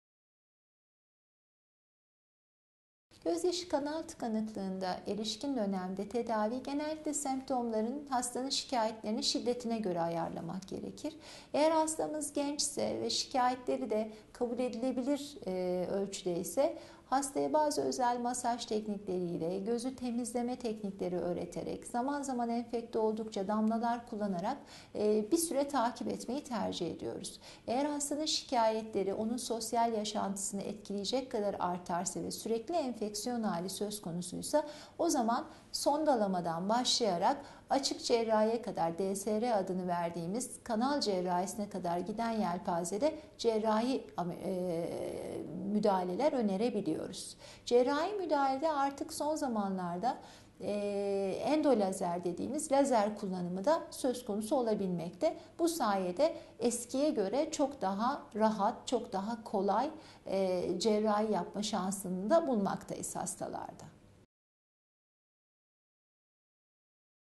Gözyaşı kanalı tıkanıklığı nasıl tedavi edilir?
Acıbadem Maslak Hastanesi, Göz Hastalıkları Uzmanı Dr. Gülbin Saltık, Göz Hastalıkları üzerine soruları yanıtlıyor.